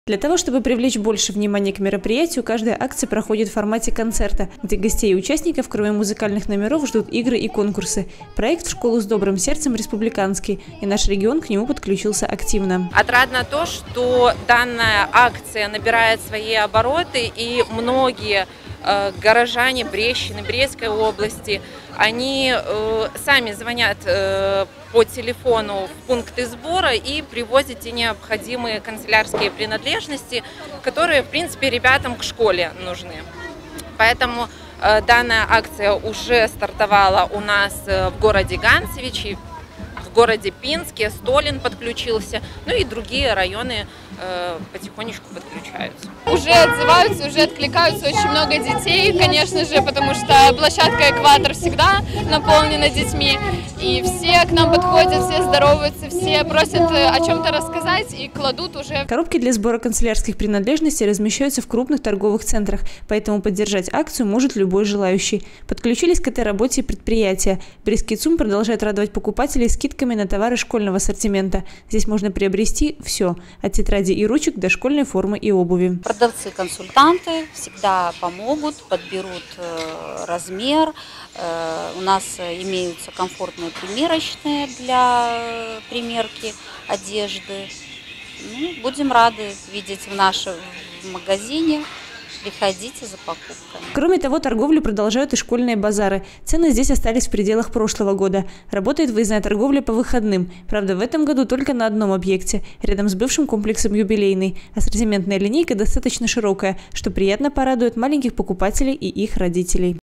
2020-08-21 г. Брест. Благотворительная акция «В школу с добрым сердцем». Новости на Буг-ТВ.
Благотворительную акцию «В школу с добрым сердцем» проводят в эти дни представители Белорусского республиканского союза молодежи. Проект призван поддержать родителей, оказавшихся в сложной жизненной ситуации. Поучаствовать в добром деле может любой желающий. Все, что нужно сделать, - это приобрести в магазине предмет из перечня школьных принадлежностей: книгу, тетради, ручки или карандаши и оставить его в специально обозначенном ящичке.